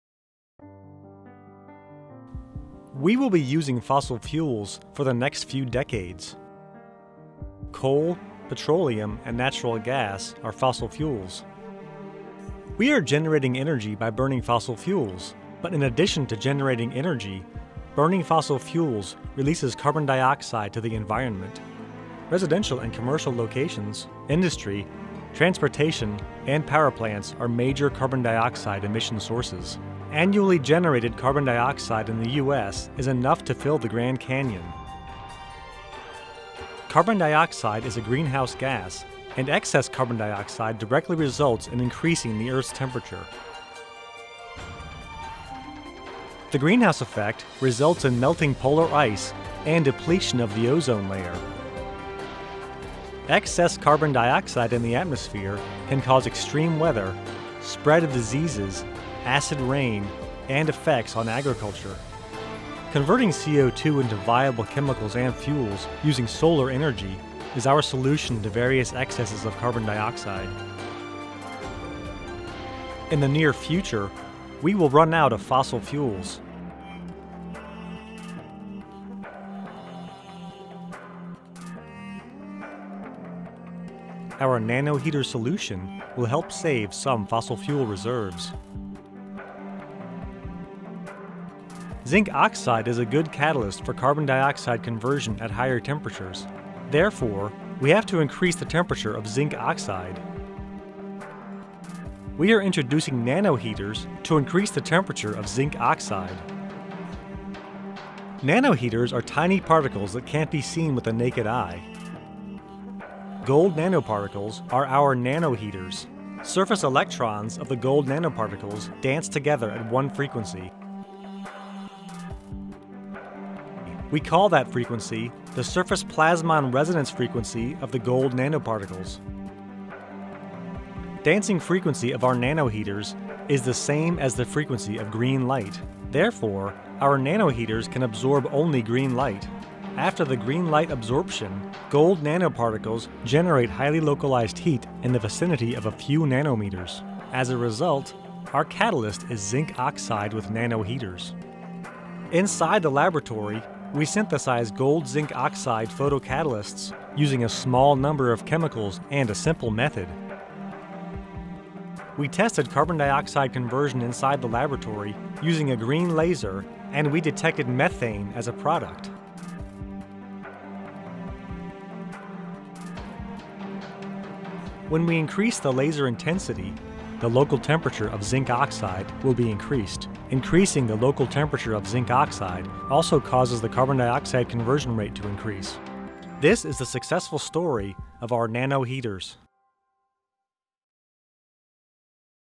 CO2 Conversion By Nano Heaters
A graduate student named Oshadha Ranasingha created this animation on the research he performed on nano heaters while working at NETL.